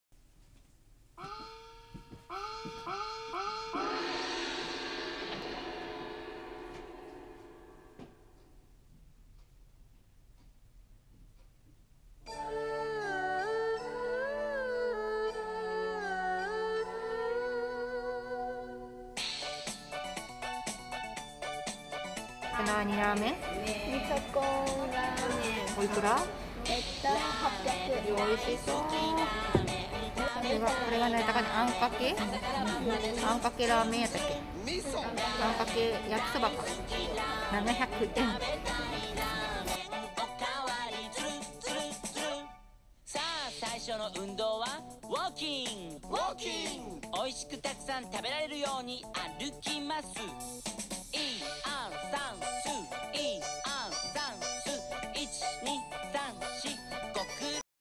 日南市☆すみよしラーメンに行ってきた🍜
宮崎県日南市のすみよしラーメンに行ってきました🍜

日南市役所のお隣にあるラーメン屋さんでメニュー豊富🎶お値段お手頃👛✨味よし立地良しで地元の人に愛される名店です。